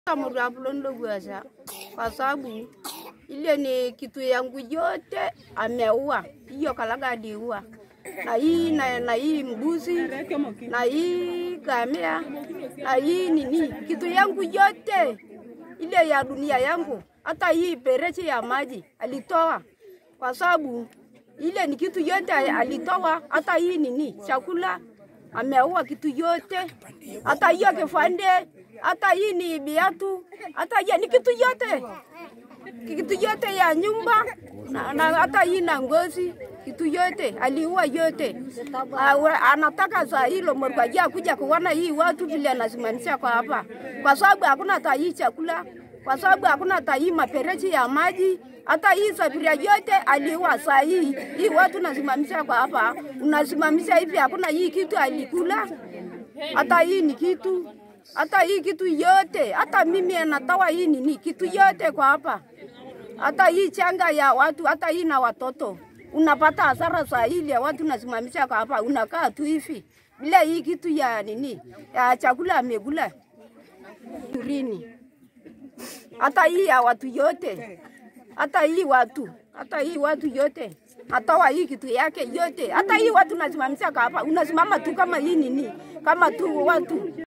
MTUSAIDIE:Hasara ya mvua Turkana.Hii 'Kalaga' kameua kila kitu yangu yote.
Wakaazi wa Turkana wakadiria hasara ya gharika  iliyosomba mali yao.